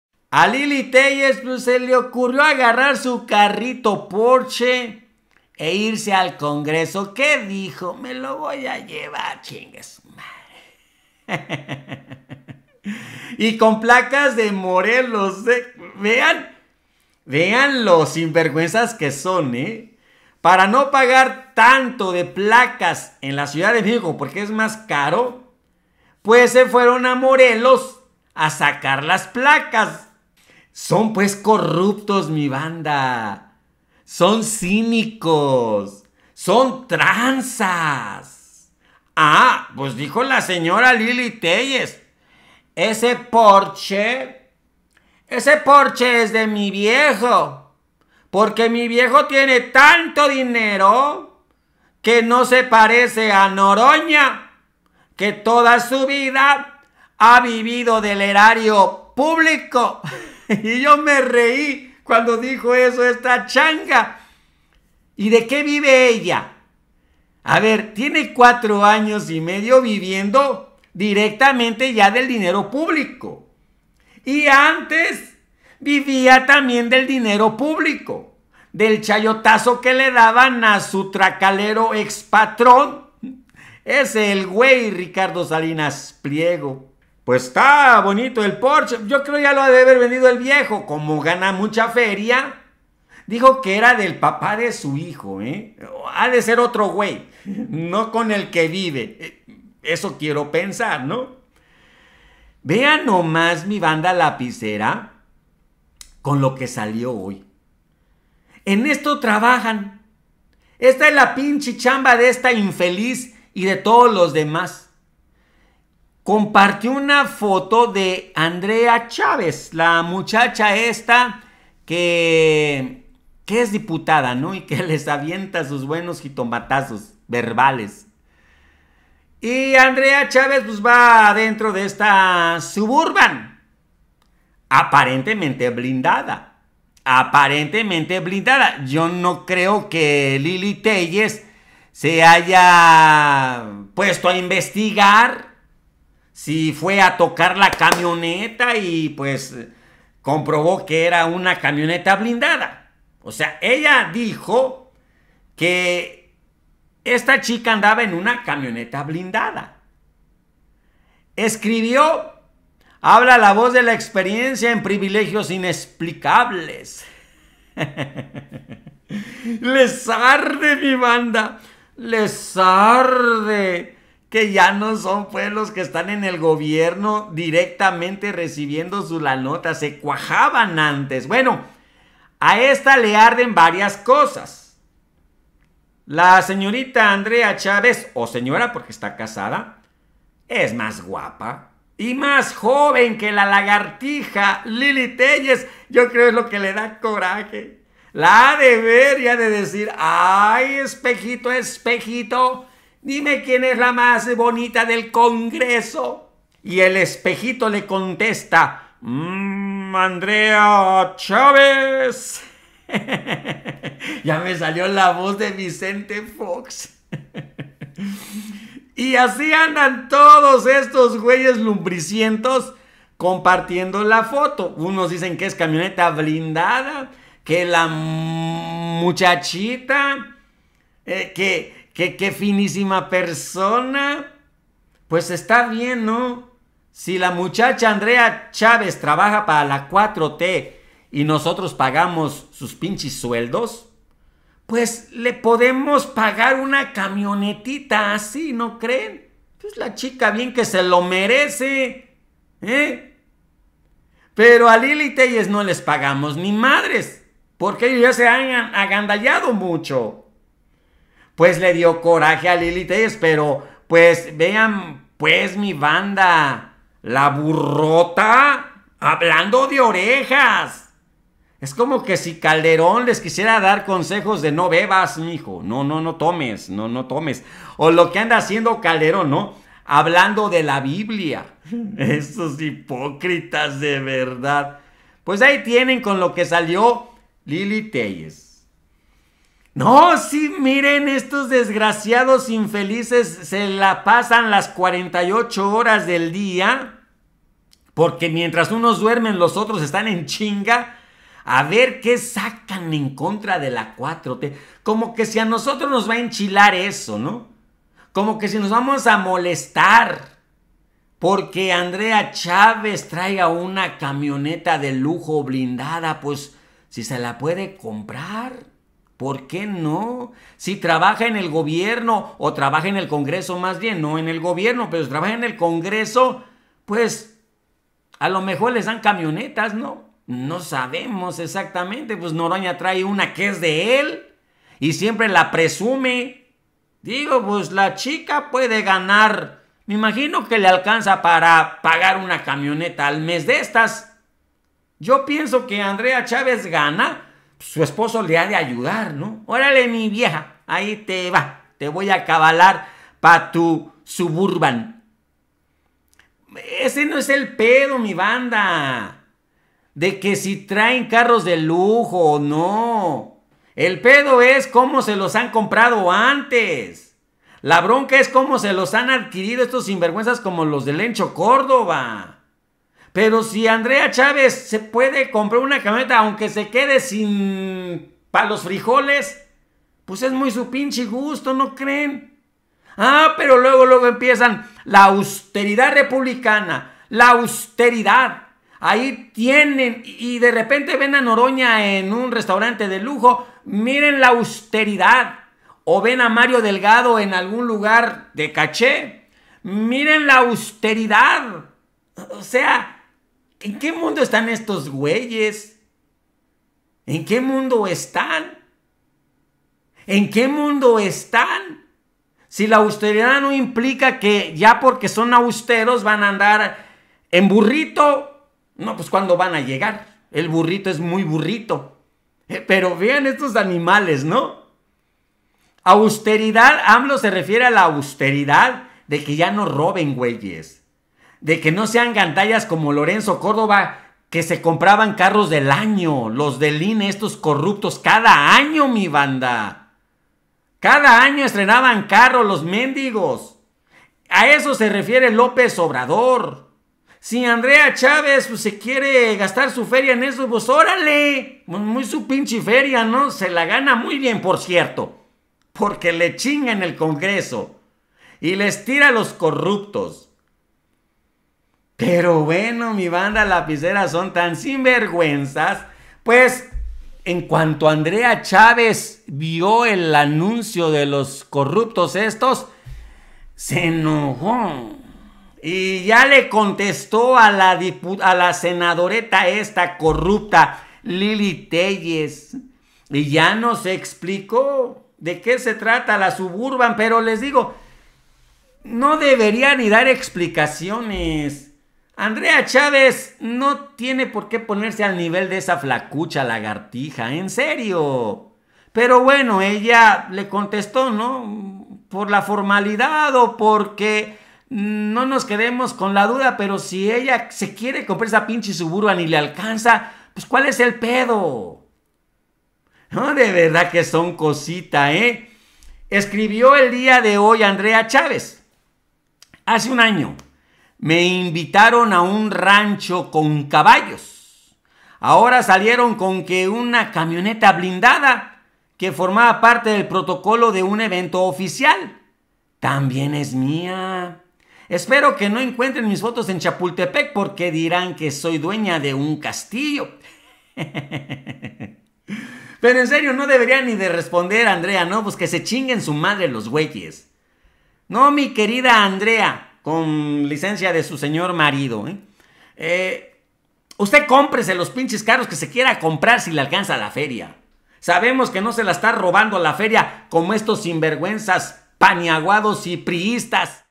Tremendo agarrón entre Andrea Chávez y la corrupta Lilly Téllez
Mis otros vídeos